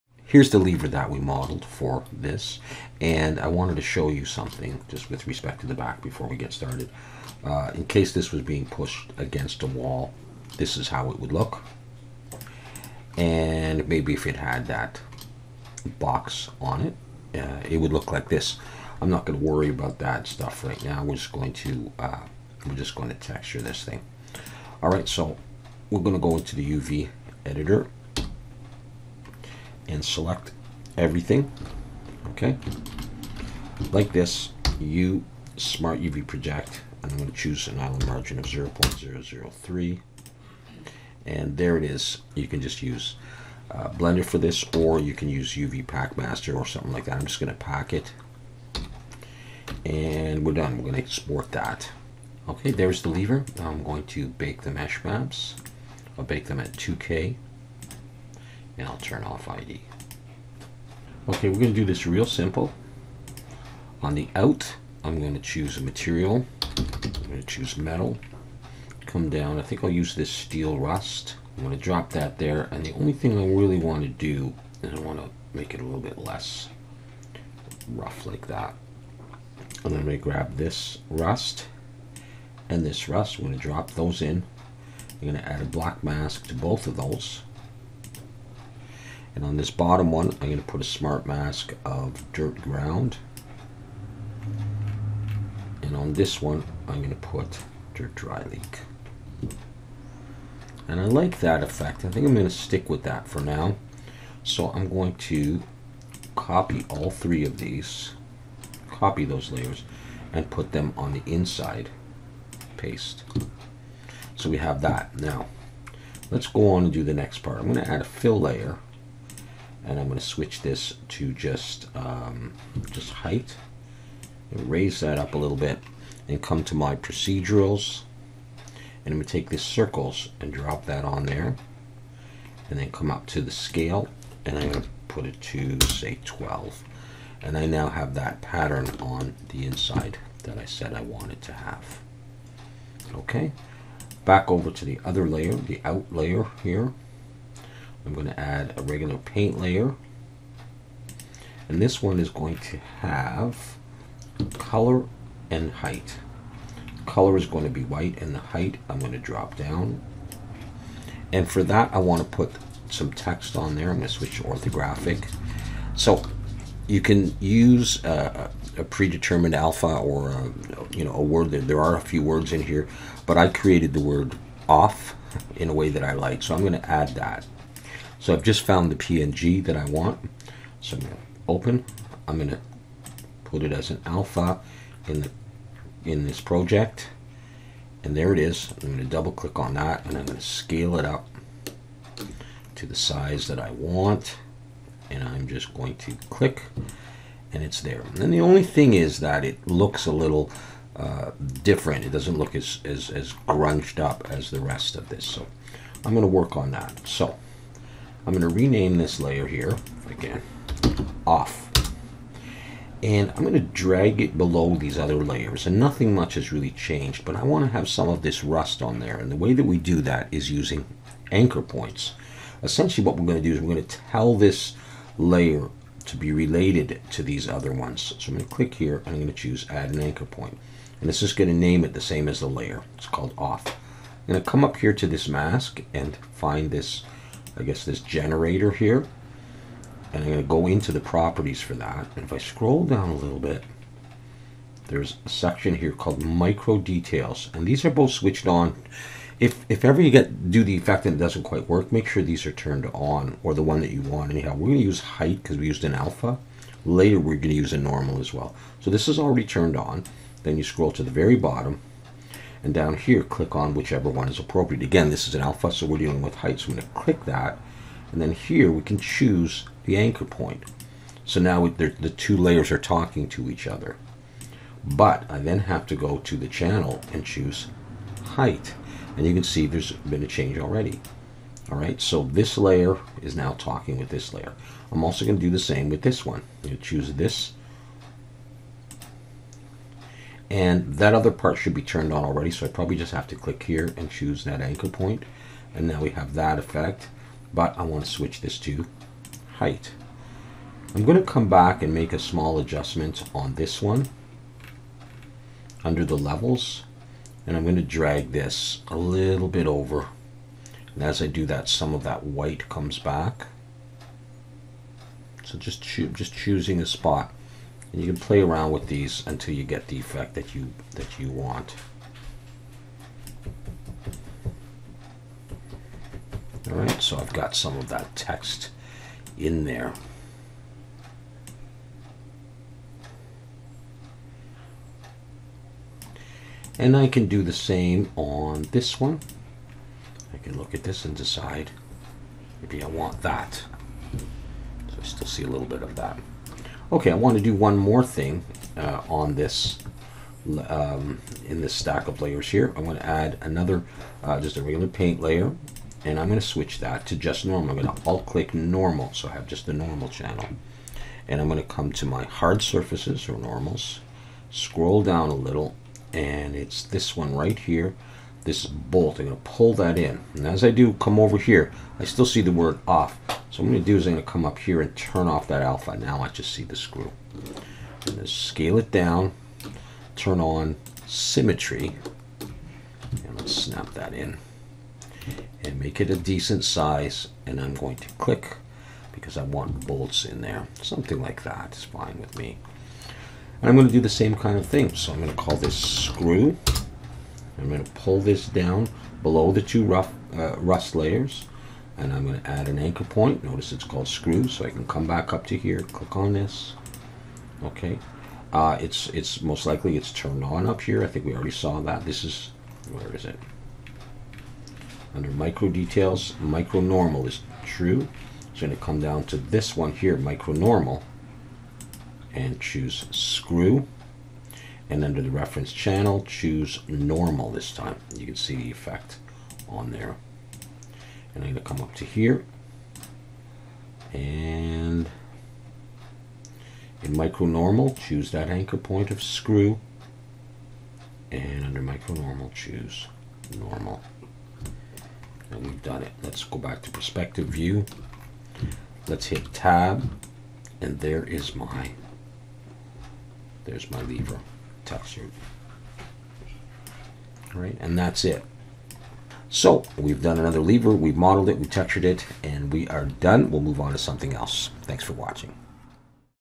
BLENDER & SUBSTANCE PAINTER: OLD LEVER TEXTURING
[Note: I will be doing a video about creating materials for this model just in Blender (ie. not in Substance Painter) so watch for that soon]

After a quick unwrap in Blender, we head over to Substance Painter to texture the old lever.  I will be using a custom alpha (really just text saved as a png) to add the word "OFF" to the lever.  And I will show how to use "anchor points" on both alphas and normals.

Part 4 (Old Lever Modeling)
Part 5 (Old Lever Texturing in Substance Painter)
Part 6 (Old Lever Texturing in Blender)